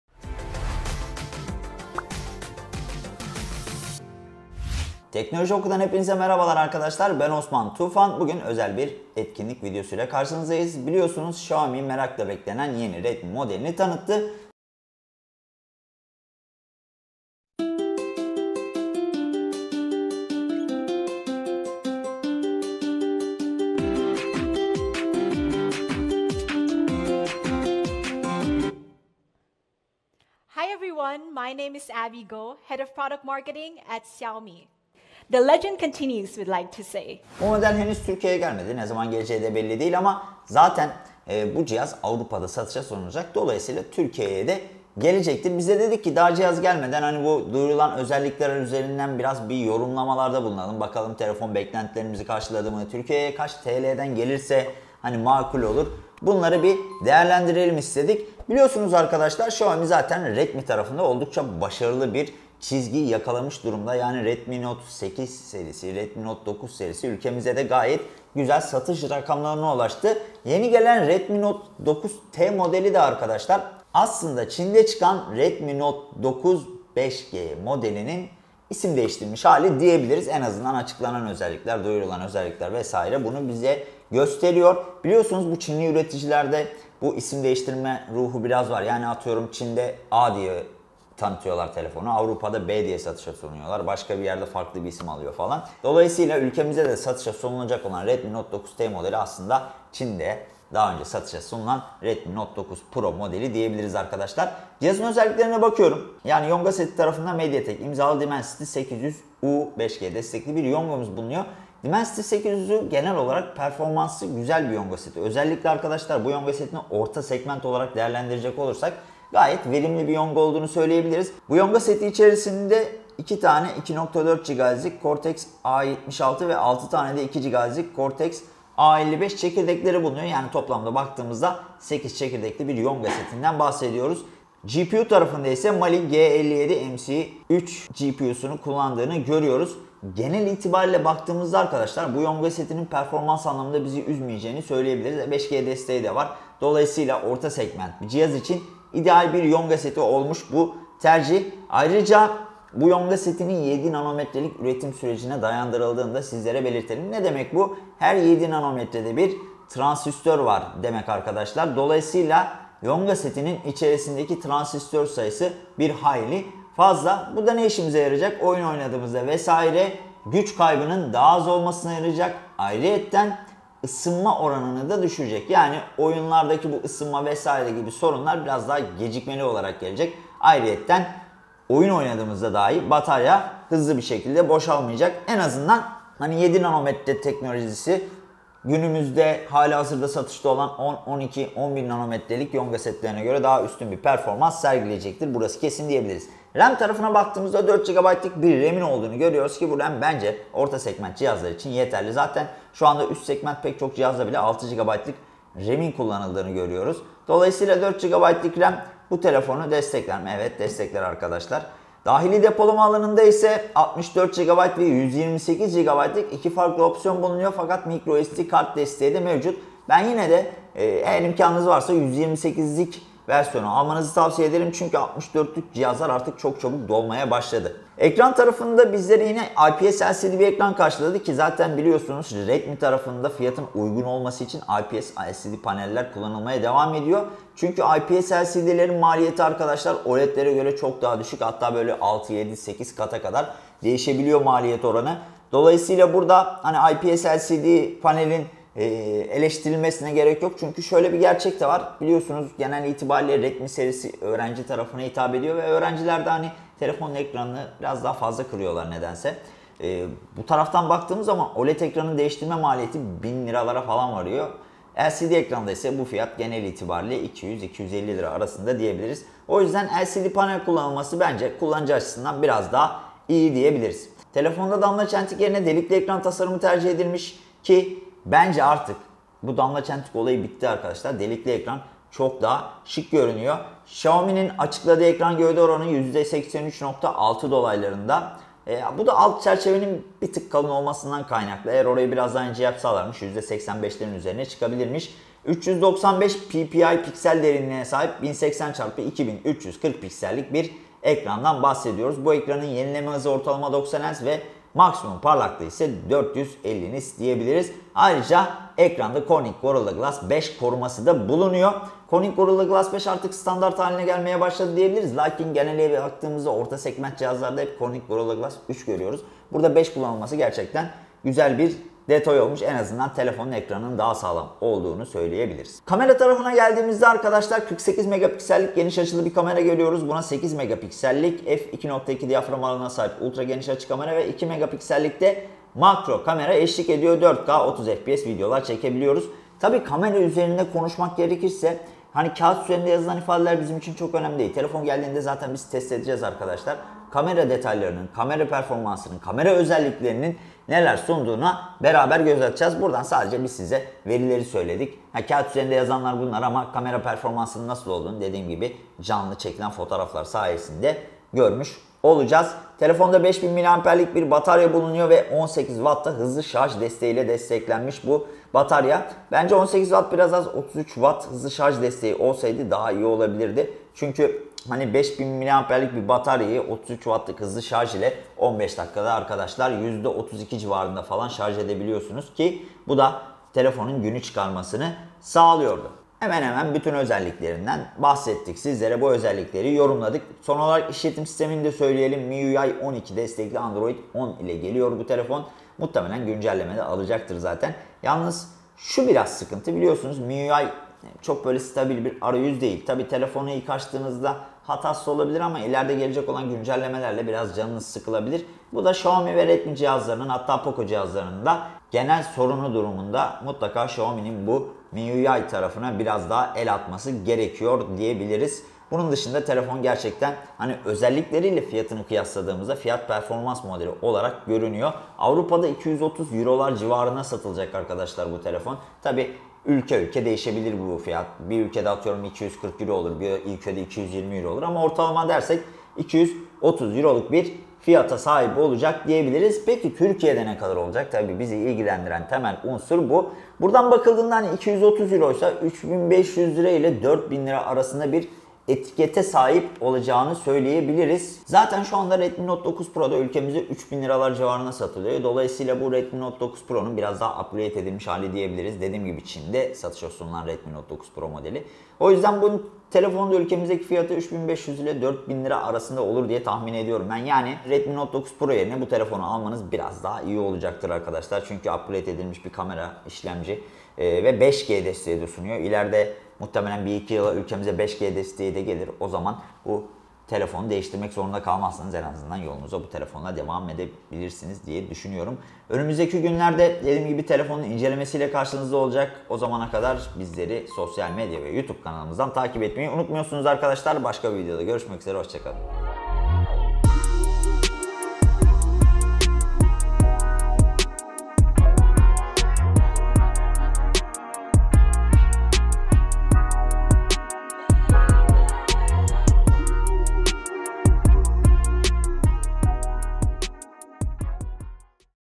Xiaomi Redmi Note 9T ile ilk sıcak temas! Yeni F/P şampiyonu!
Xiaomi yeni fiyat performans modelini Avrupa'ya getiriyor. Kısa bir süre önce Çin'de satışa çıkan Xiaomi Redmi Note 9 Pro 5G modeli isim değiştirerek Avrupa'nın yolunu tuttu. Avrupa ve dolayısıyla Türkiye'de Redmi Note 9T ismiyle satılacak olan modelin özelliklerini bu videoda değerlendirdik.

İzleyeceğiniz videoda Xiaomi Redmi Note 9T özellikleri, Avrupa fiyatı, Türkiye fiyatı, hali hazırda ülkemizde satılmakta olan Redmi Note 9 Pro modelinden farklı gibi detaylar yer alıyor. Şimdiden iyi seyirler dileriz. Telefon hakkındaki yorumlarınızı bizlerle paylaşırsanız mutlu oluruz. Sizlerin yorumları bizler için önemli.